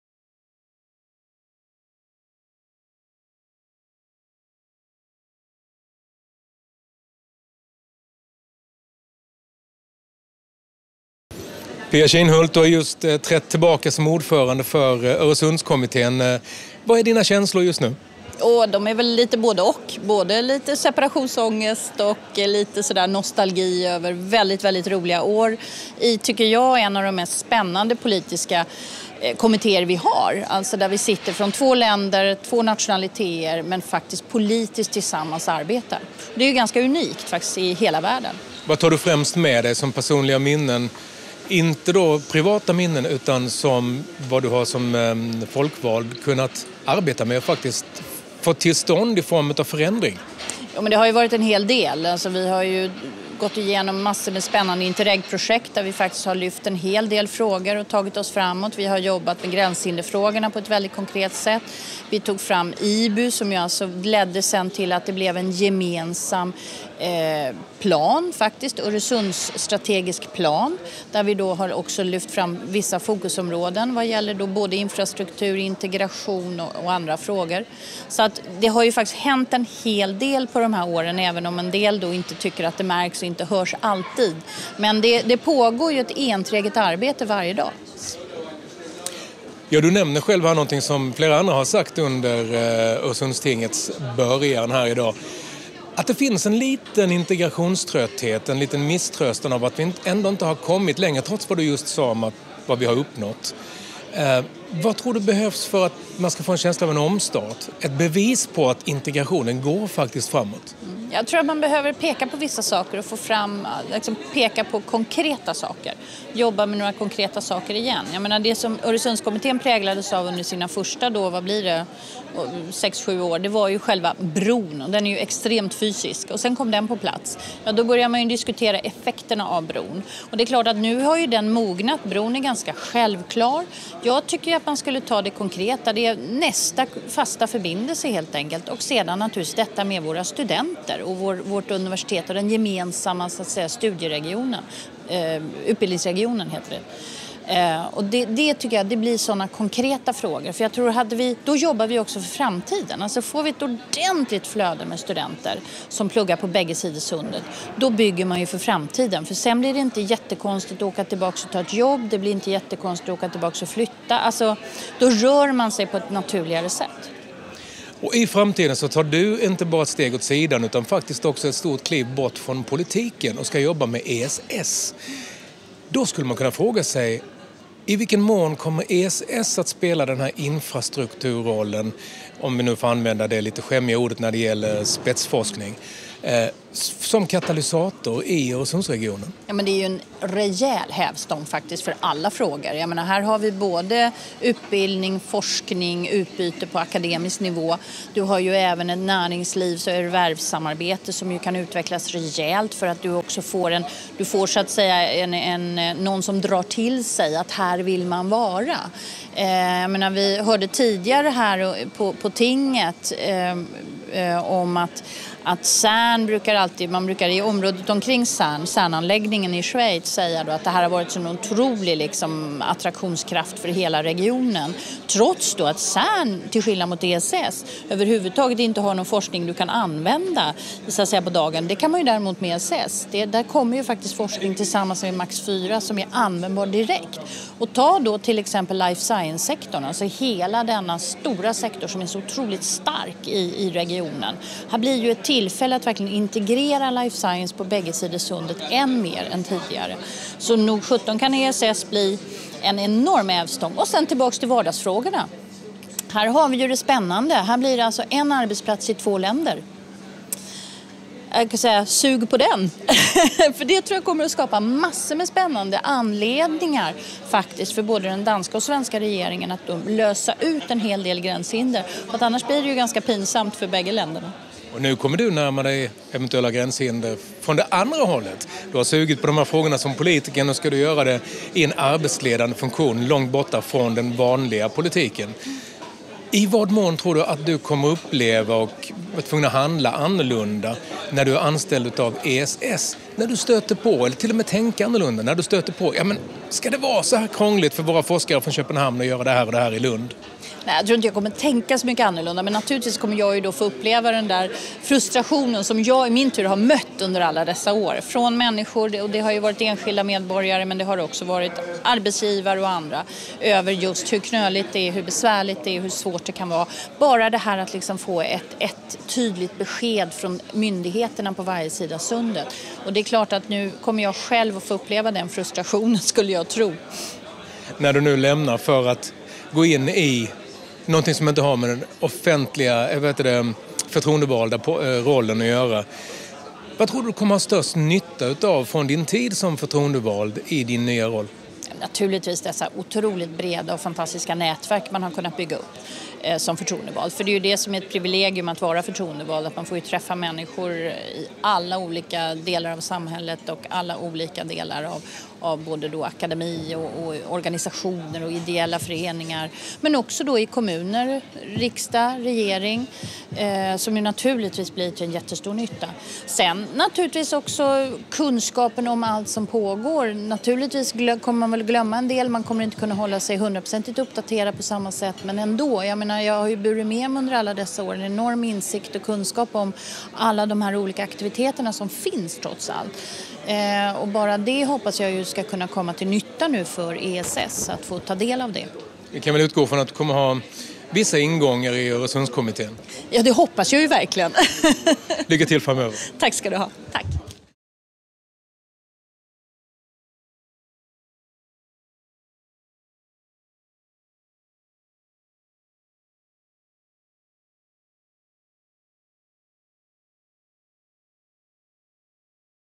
Interview med Pia Kinhult, ordförande i Öresundskomiteen 2014 - Öresundsting 2014
Pia Kinhult, ordförande i Öresundskomiteen 2014, nyligen avgängen regionstyrelseordförande i Region Skåne.

Öresundstinget den 6. november 2014, på MS Tycho Brahe.